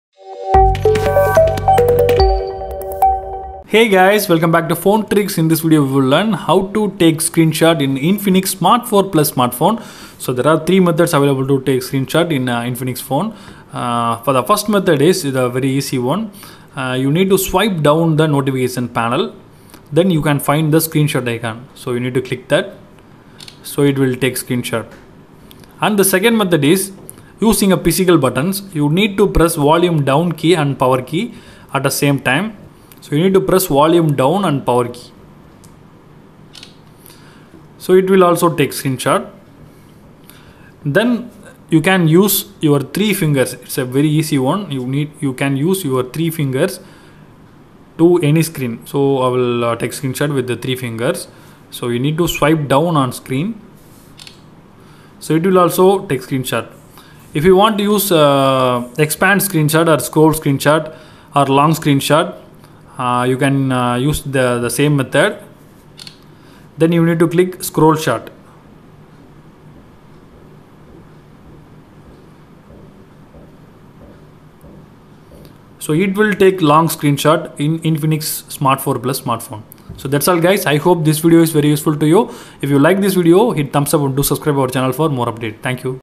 How to take Screenshot in Infinix smart 4 plus
Hey Friends  ! Checkout this new video of How to take Screenshot in Infinix smart 4 plus